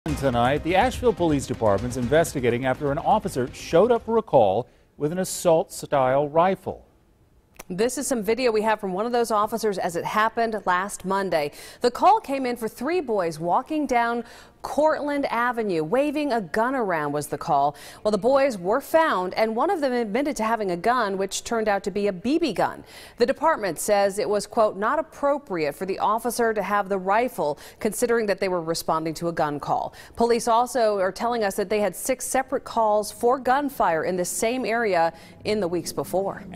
Asheville PD looking into actions of officer who brought rifle to call
The Asheville Police Department is investigating after an officer showed up to a call with an assault style rifle.